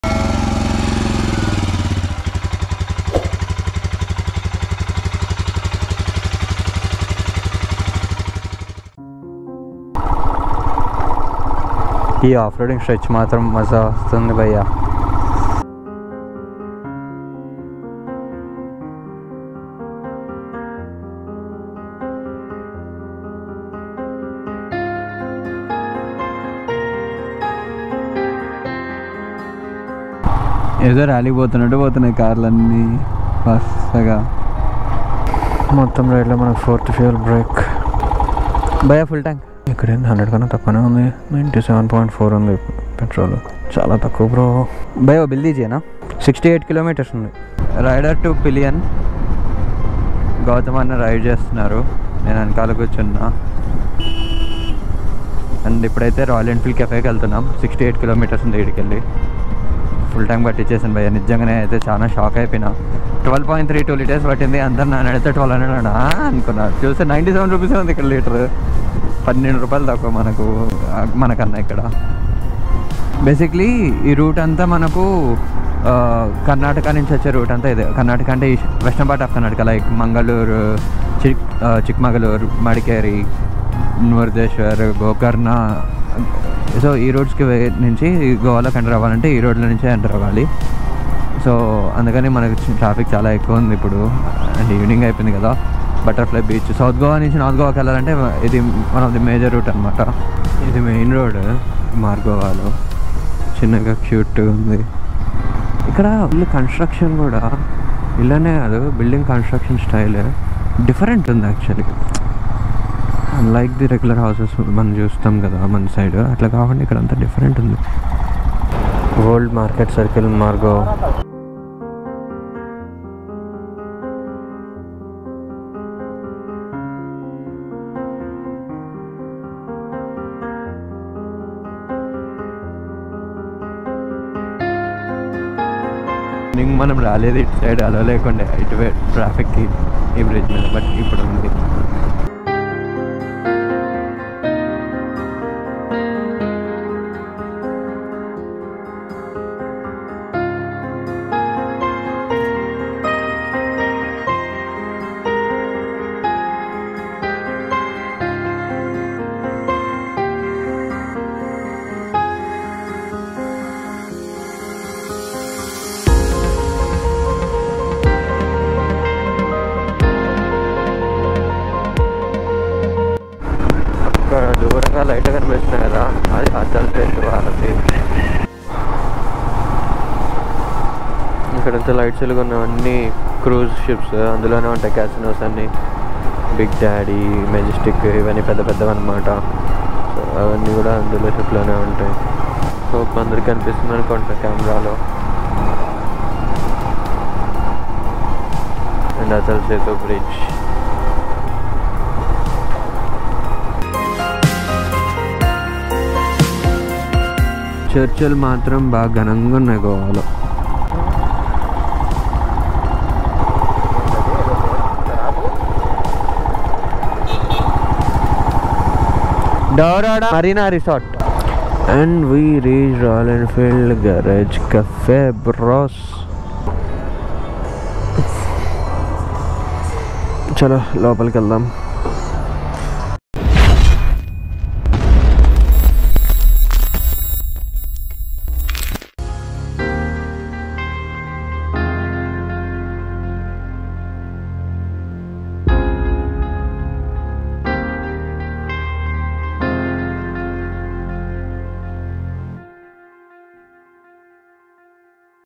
Riding on Atul Sethu Varadhi | World's 12th Longest Sea Bridge | Goa Ride Day 2 | Moto Sanchari
This video is about me riding to northgoa from southgoa via atulsetu in night enjoying the beautiful views of Atul Setu Bridge.

Watch the video & explore as I go.